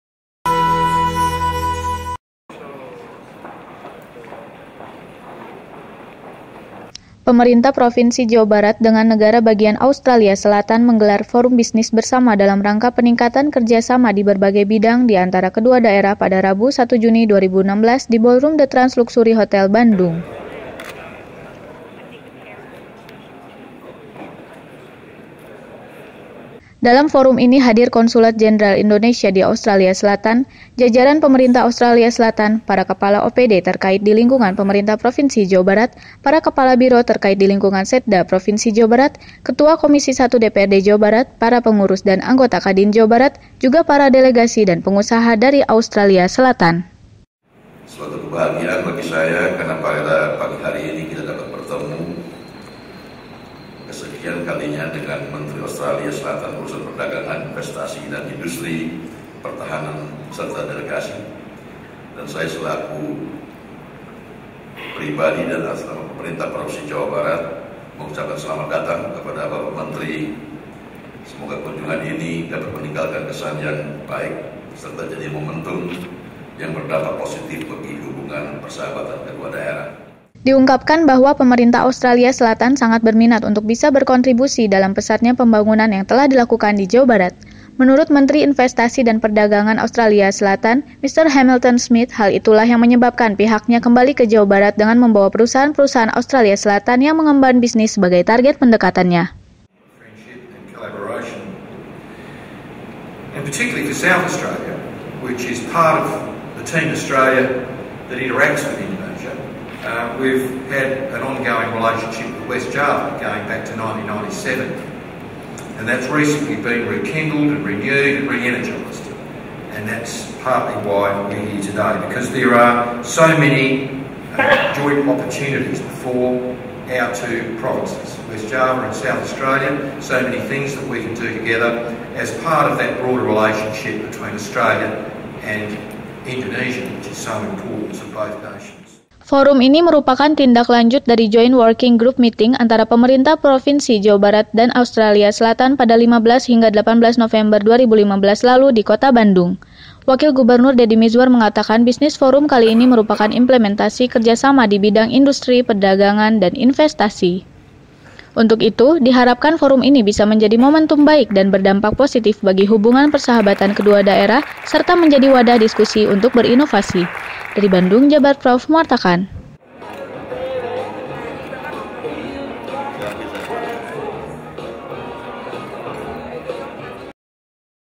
Jabarprov TV - Forum Bisnis Bersama Jawa Barat & Autralia Selatan
Wakil Gubernur Jawa Barat menerima kunjungan kerja Menteri Perdagangan dan Investasi Australia Selatan pada Rabu 1 Juni 2016 di The Trans Luxury Hotel. Adapun tujuan Menteri Perdagangan dan Investasi Australia Selatan membahas kerjasama dengan Jawa Barat di Berbagai Bidang.